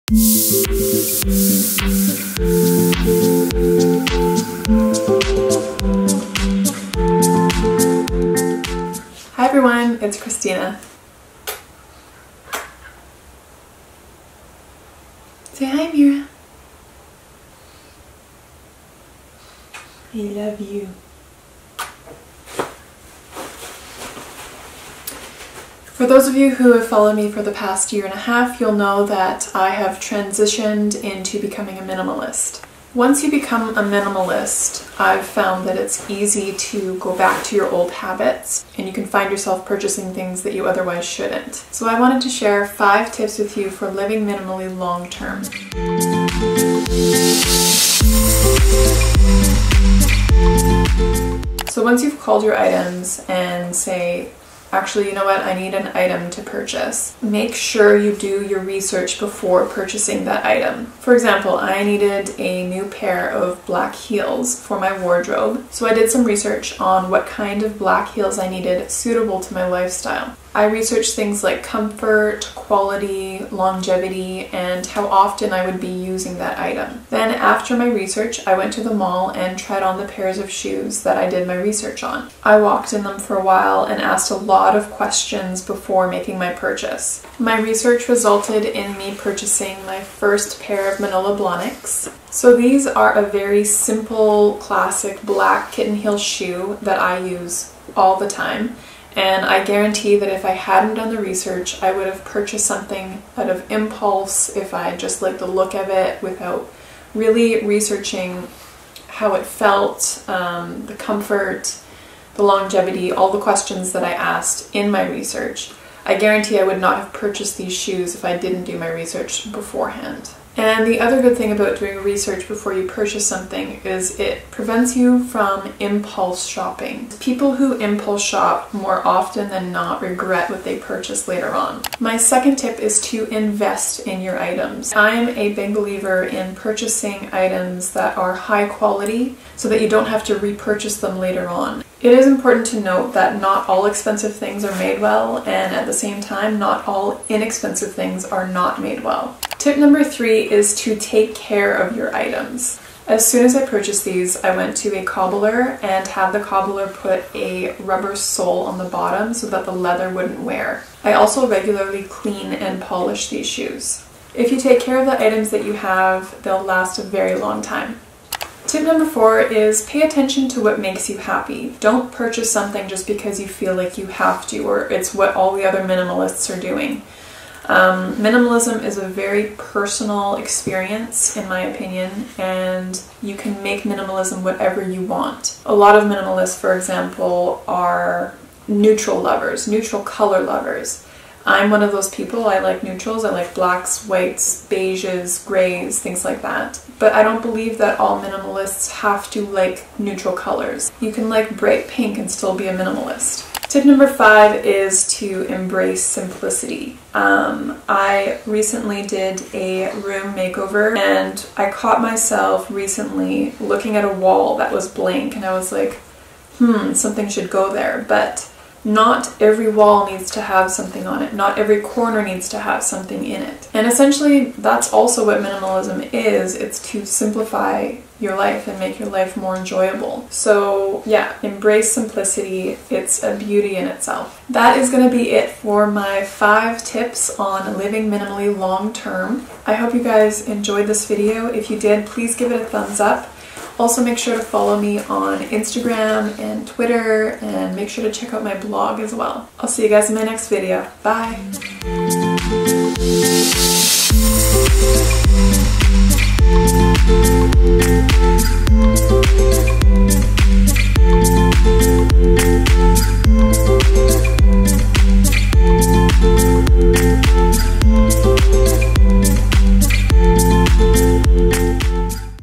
5 TIPS TO LIVE MINIMALLY LONG-TERM l KristinaDanielle
Hi Everyone!

Today's video is about how to live minimally long-term.  There has been a huge wave of people wanting to try minimalism.  For many, minimalism is just a phase, so these tips will help those make minimalism more permanent in their lives.

Check out my blog post on how to live minimal long term

xo,
K